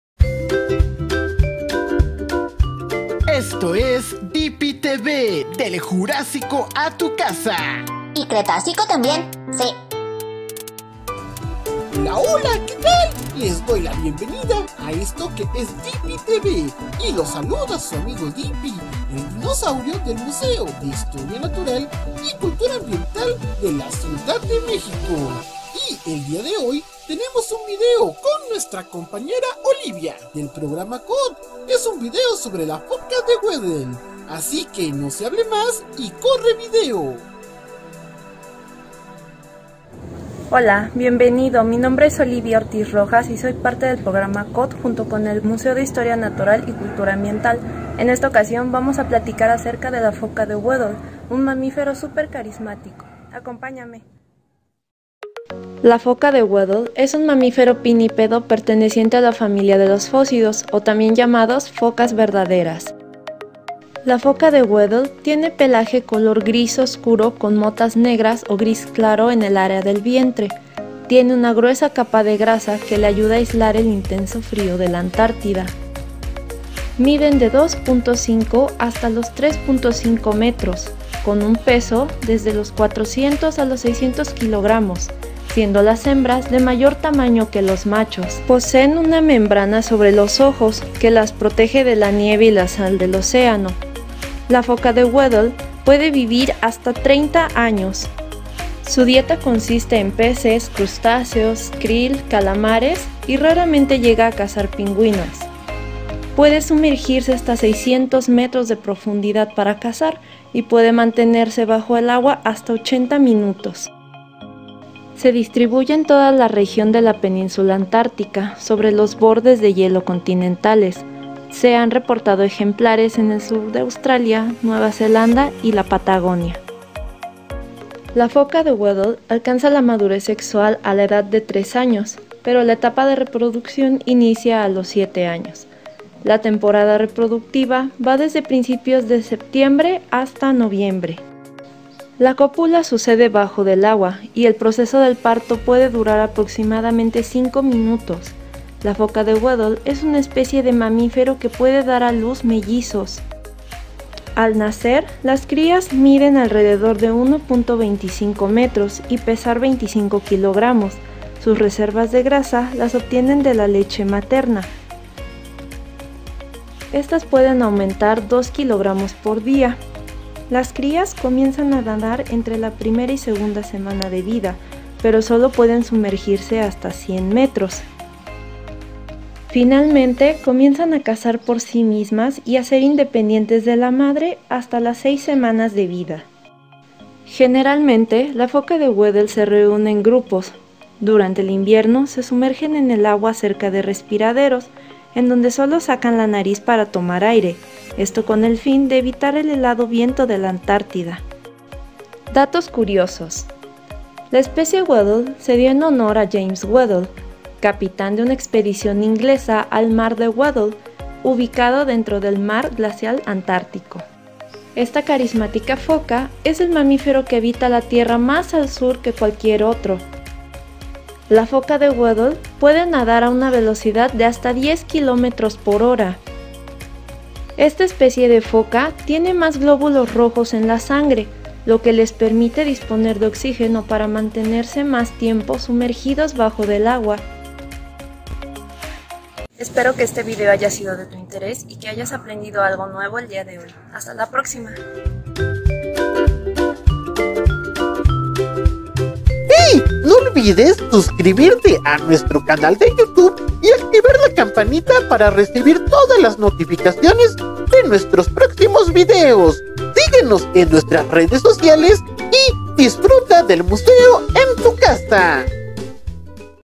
Dippy Tv Foca de Weddell
Dippy Tv te invita te invita a ver el siguiente video sobre Foca de Weddell, a cargo de Olivia, COT.

En colaboración de Servicio Nacional de Empleo, CDMX.